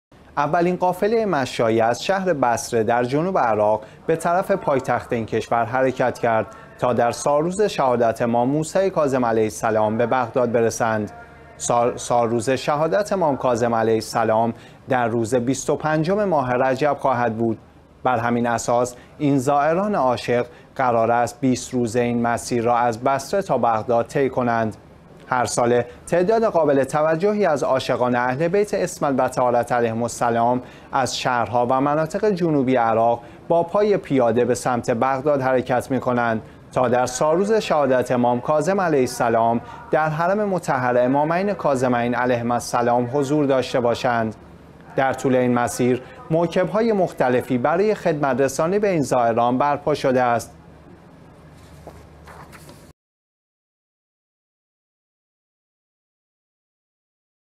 حرکت اولین قافله مشایه از بصره به بغداد برای شرکت در مراسم شهادت امام کاظم علیه السلام
اولین قافله مشایه از شهر بصره در جنوب عراق به طرف پایتخت این کشور حرکت کرد تا در سالروز شهادت امام موسی کاظم علیه السلام به بغداد برسند.
سالروز شهادت امام کاظم علیه السلام در روز بیست و پنجم ماه رجب خواهد بود بر همین اساس این زائران عاشق، قرار است 20 روزه این مسیر را از بصره تا بغداد طی کنند.
هر ساله تعداد قابل توجهی از عاشقان اهل بیت عصمت و طهارت علیهم السلام از شهرها و مناطق جنوبی عراق با پای پیاده به سمت بغداد حرکت می کنند تا در سالروز شهادت امام کاظم علیه السلام، در حرم مطهر امامین کاظمین علیهما السلام حضور داشته باشند.
در طول این مسیر، موکب های مختلفی برای خدمت رسانی به این زائران برپا شده است.